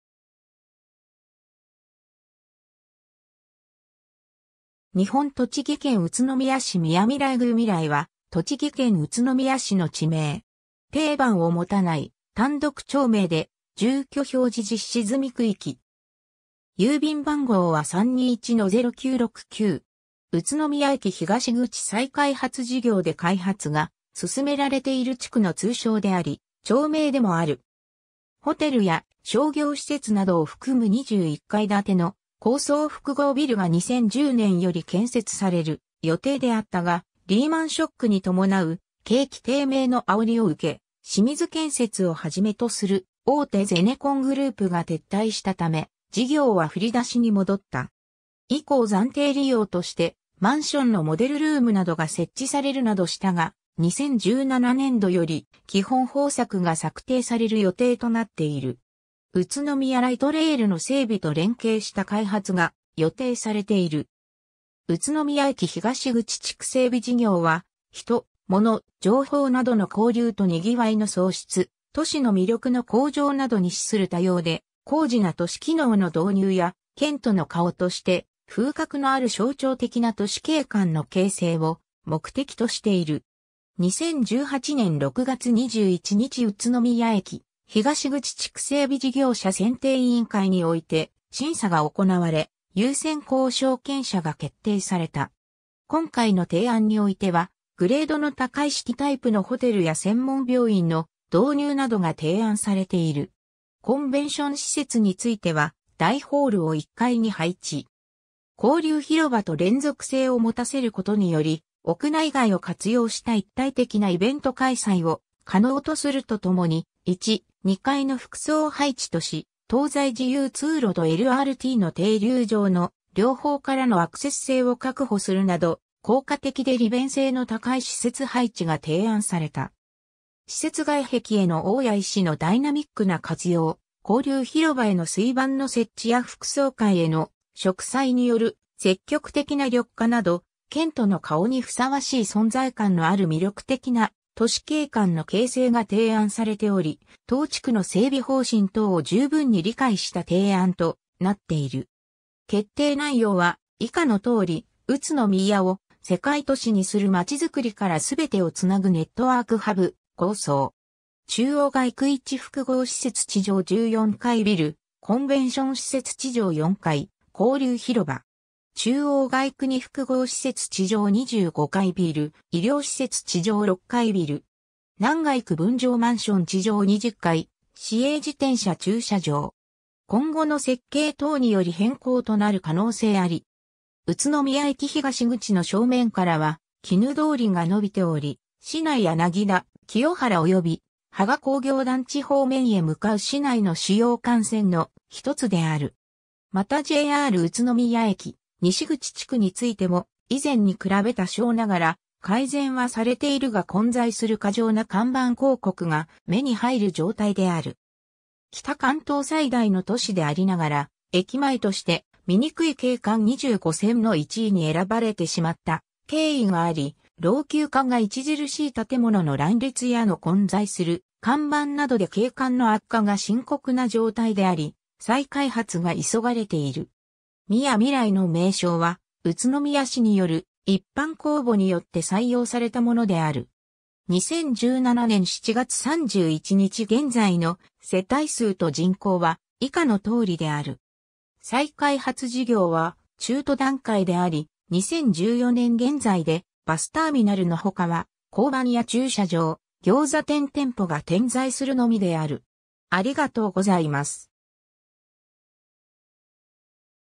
宮みらい
2009年制定の町・字
日本 」 栃木県 」 宇都宮市 」 宮みらい 宮みらい（みやみらい）は栃木県宇都宮市の地名。
丁番を持たない単独町名で、住居表示実施済み区域。
郵便番号は321-0969（宇都宮東郵便局管区）。
 宇都宮駅東口再開発事業で開発が進められている地区の通称であり、町名でもある。
ホテルや商業施設などを含む21階建ての高層複合ビルが2010年より建設される予定であったが、リーマンショックに伴う景気低迷のあおりを受け、清水建設をはじめとする大手ゼネコングループが撤退したため、事業は振り出しに戻った。
以降暫定利用としてマンションのモデルルームなどが設置されるなどしたが、2017年度より基本方策が策定される予定となっている。
宇都宮ライトレールの整備と連携した開発が予定されている。
 宇都宮駅東口地区整備事業は「人・もの・情報」などの交流と賑わいの創出、都市の魅力の向上などに資する多様で高次な都市機能の導入や、県都の顔として風格のある象徴的な都市景観の形成を目的としている。
 2018年6月21日-宇都宮駅東口地区整備事業者選定委員会において審査が行われ、優先交渉権者が決定された。
 今回の提案においては、グレードの高いシティタイプのホテルや専門病院の導入などが提案されている。
 コンベンション施設については大ホールを１階に配置。
交流広場と連続性を持たせることにより、屋内外を活用した一体的なイベント開催を可能とするとともに、１、２階の複層配置とし東西自由通路とLRTの停留場の両方からのアクセス性を確保するなど、効果的で利便性の高い施設配置が提案された。
 施設外壁への大谷石のダイナミックな活用、交流広場への水盤の設置や複層階への植栽による積極的な緑化など、県都の顔にふさわしい存在感のある魅力的な都市景観の形成が提案されており当地区の整備方針等を十分に理解した提案となっている。
 決定内容は以下の通り 「うつのみや」を世界都市にするまちづくり -すべてをつなぐネットワークハブ構想。
 中央街区① 複合施設 地上14階ビル、コンベンション施設 地上4階(2000人規模収容ホール)、交流広場。
 中央街区② 複合施設 地上25階ビル(県内最高層)、医療施設 地上6階ビル。
 南街区 分譲マンション 地上20階、市営自転車駐車場。
 ※今後の設計等により変更となる可能性あり。
 宇都宮駅東口の正面からは鬼怒通り（主要地方道宇都宮向田線。県道としての指定は東宿郷交差点から東であり、駅東口から東宿郷交差点までは市道。）が延びており、市内柳田・清原および芳賀工業団地方面へ向かう市内の主要幹線の一つである。
 またJR宇都宮駅西口地区についても、以前に比べ多少ながら改善はされているが混在する過剰な看板広告が目に入る状態である。
 北関東最大の都市でありながら駅前として「みにくい景観２５選」の１位に選ばれてしまった経緯があり、老朽化が著しい建物の乱立やの混在する看板などで景観の悪化が深刻な状態であり再開発が急がれている。
 「宮みらい」の名称は宇都宮市による一般公募によって採用されたものである。
 2017年（平成29年）7月31日現在の世帯数と人口は以下の通りである。
 再開発事業は中途段階であり、2014年現在でバスターミナルのほかは交番や駐車場、餃子店店舗が点在するのみである。